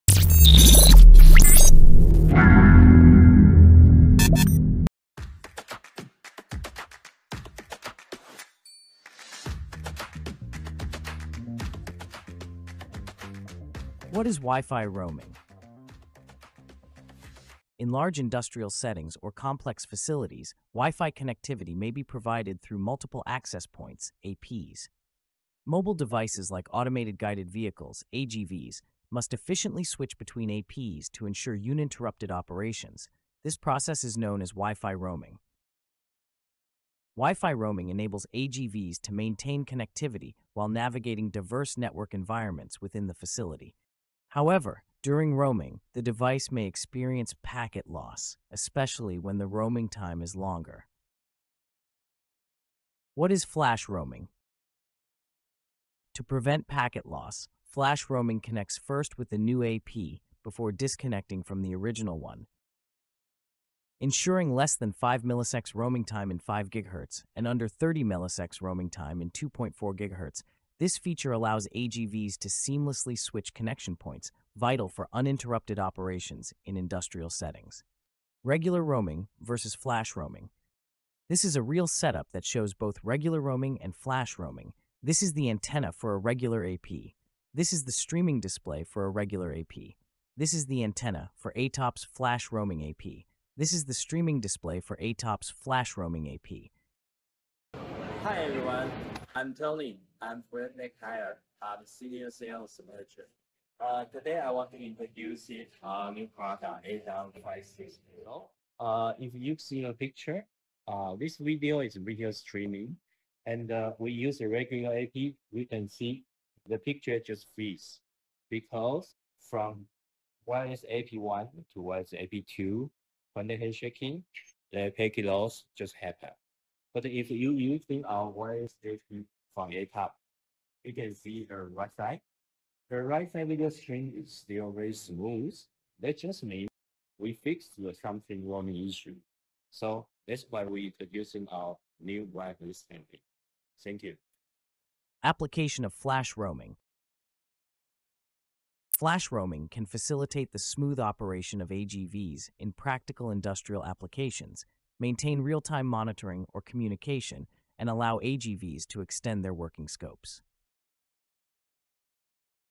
Punto de Acceso Wi-Fi Flash Roaming de ATOP
La tecnología Flash Roaming de ATOP permite un roaming Wi-Fi rápido y confiable para grandes sitios industriales y almacenes automatizados. Conoce las diferencias entre el roaming Wi-Fi regular y Flash Roaming, y cómo Flash Roaming beneficia tu automatización.

¿Tienes dudas sobre qué producto utilizar en tu proyecto? 🤔

¡Visita nuestros blogs! 🤓